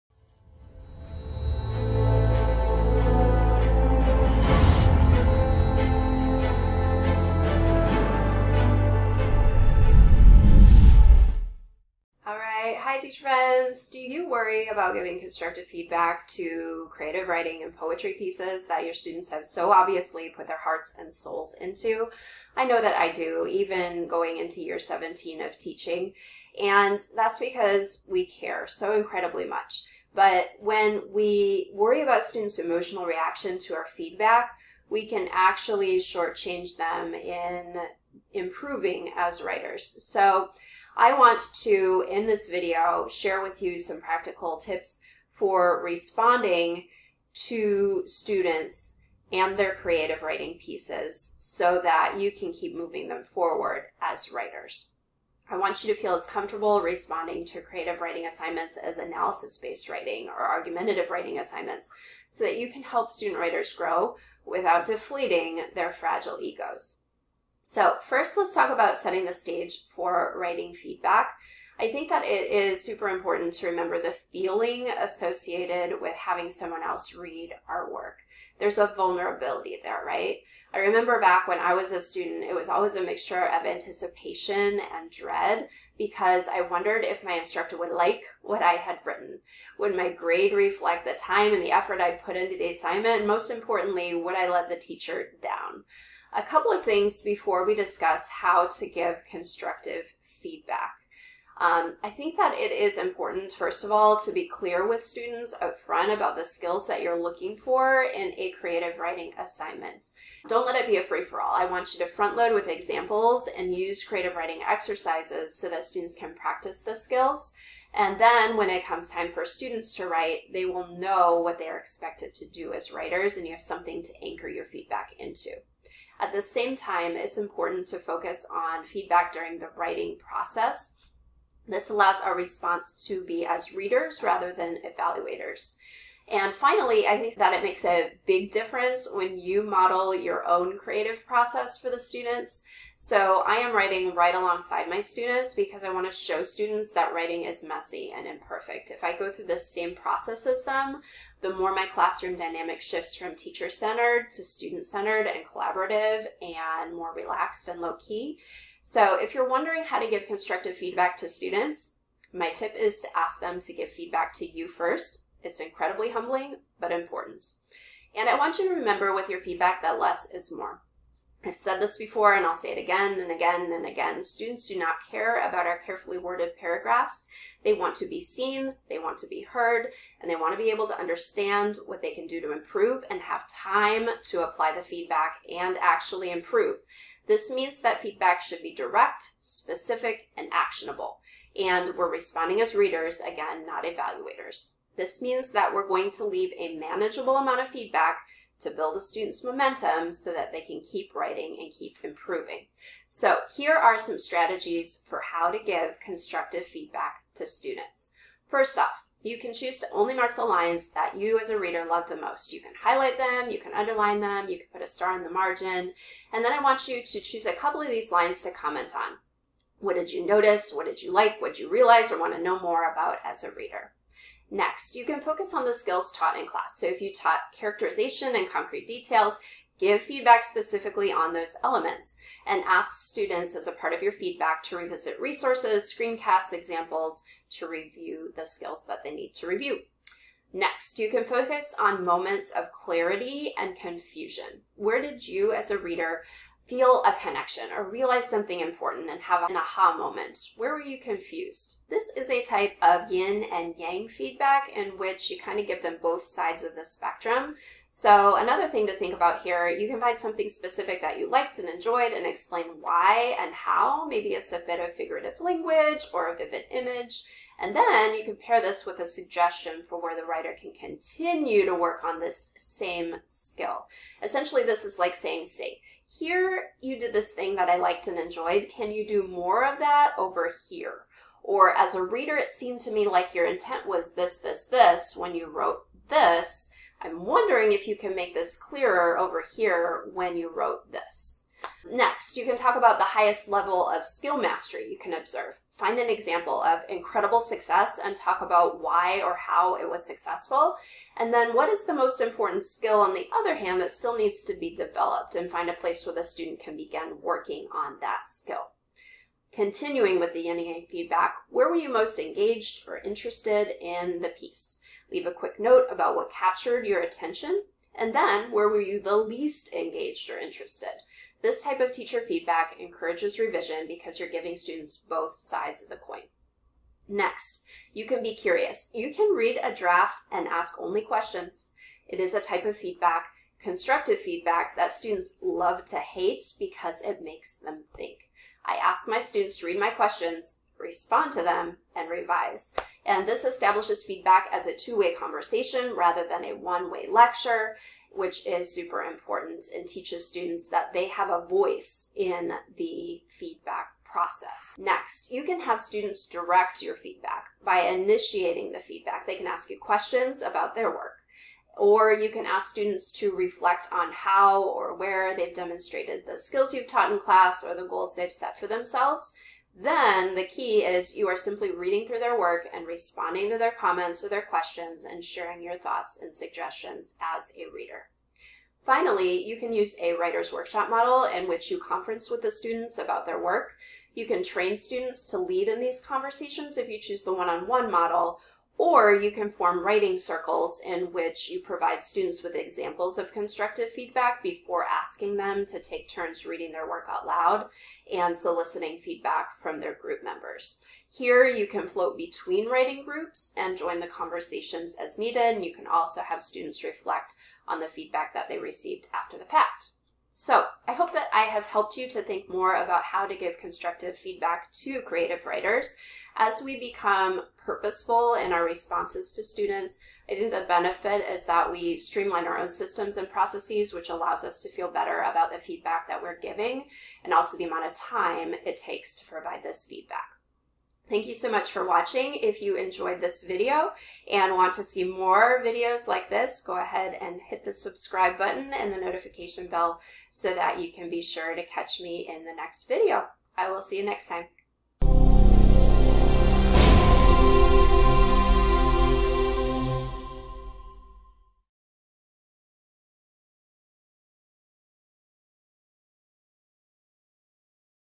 How to Give Effective Feedback to Your Students on Creative Writing Assignments
Giving effective feedback to your student writers can be challenging, especially when it comes to teacher feedback on creative writing pieces that tend to be heartfelt and deeply personal. This video will give you practical strategies that will teach you how to give teacher feedback to student writers. I want you to feel as comfortable responding to creative writing assignments as analysis based writing or argumentative writing assignments so that you can help student writers grow without deflating their fragile egos.